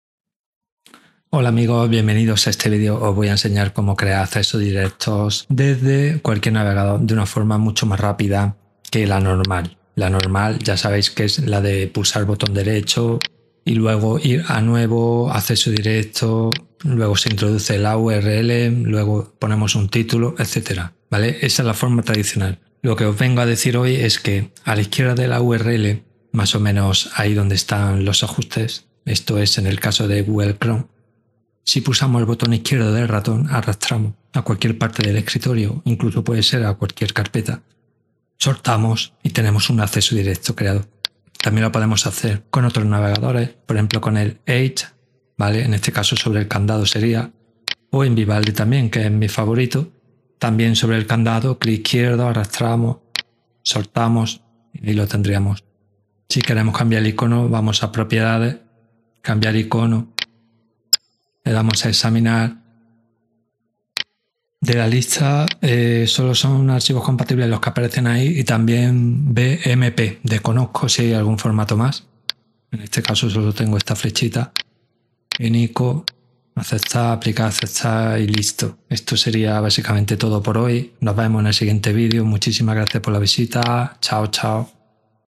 Truco crear acceso directo en el escritorio a una página web en un segundo 🎯
🎯 Tutorial sobre cómo crear accesos directos a páginas web en el escritorio. Es un método sencillo y útil que funciona en la mayoría de los navegadores.

**Resumen del tutorial:**

1. **Abrir la página web:** Abre la página web que deseas convertir en acceso directo en tu navegador preferido.
2. **Arrastrar el icono:** Mantén pulsado el icono que aparece junto a la URL de la página (generalmente un candado) y arrástralo al escritorio.
3. **Acceso directo creado:** Suelta el icono en una zona libre del escritorio y se creará automáticamente un acceso directo a la página web.

**Aclaraciones importantes:**

* **Icono variable:** El icono que debes arrastrar puede variar según el navegador, pero siempre se encuentra junto a la URL.
* **Navegador predeterminado:** Al hacer clic en el acceso directo, la página web se abrirá en el navegador predeterminado de tu sistema operativo Windows.

**Información adicional:**

* **Otros métodos:** Existen otros métodos para crear accesos directos a páginas web, como usar las opciones del menú del navegador o crear un acceso directo manualmente.
* **Personalización:** Puedes cambiar el nombre y el icono del acceso directo haciendo clic derecho sobre él y seleccionando "Propiedades".

Espero que este resumen y las aclaraciones adicionales sean útiles para los usuarios. Gracias por la visita